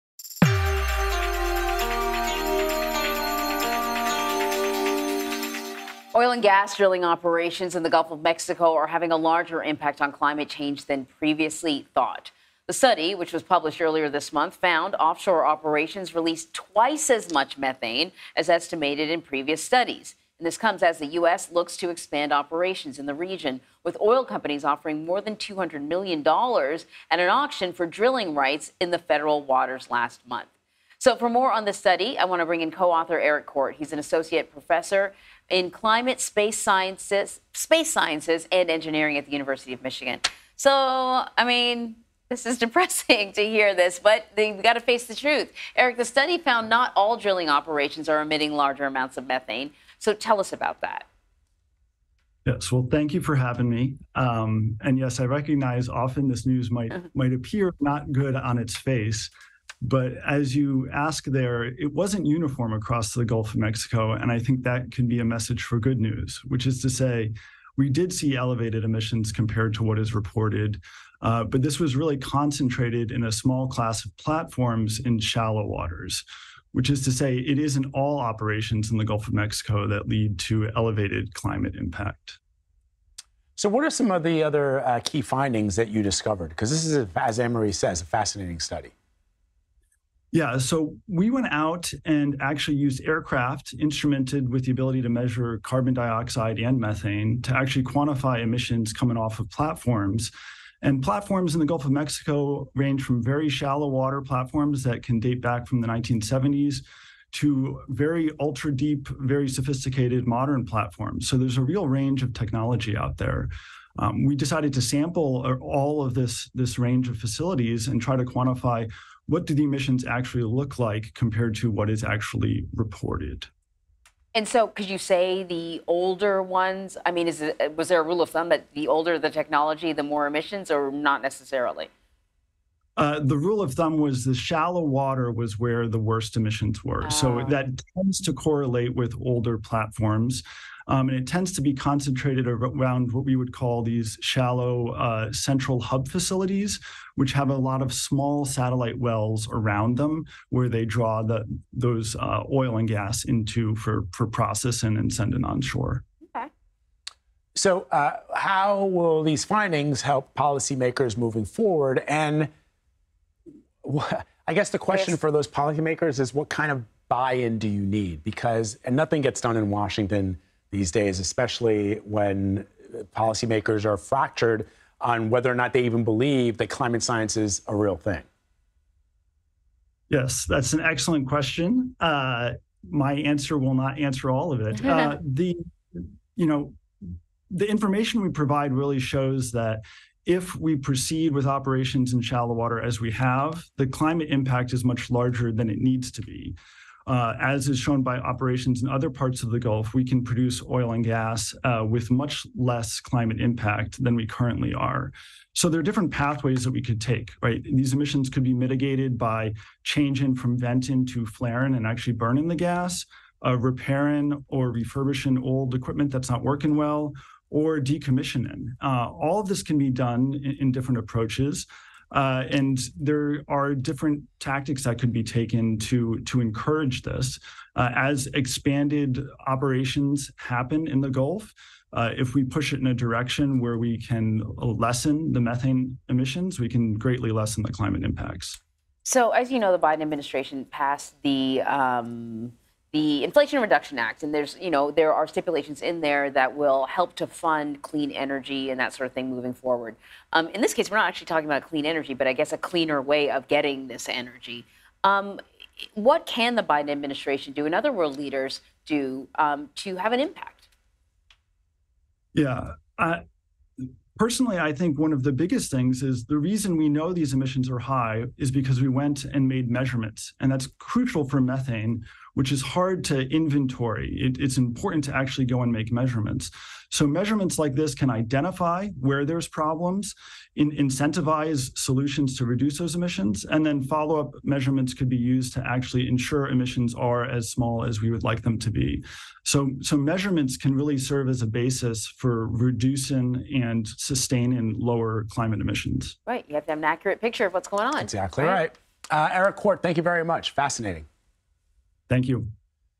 Methane emissions from Gulf of Mexico oil drilling higher than estimated, study finds
A study published in April found oil and gas drilling operations in the Gulf of Mexico released twice as much methane as estimated in previous studies. Eric Kort, co-author of the study and associate professor of climate, space sciences and engineering at the University of Michigan, joined CBS News to discuss the data and ways to mitigate the damage caused by this climate-changing gas.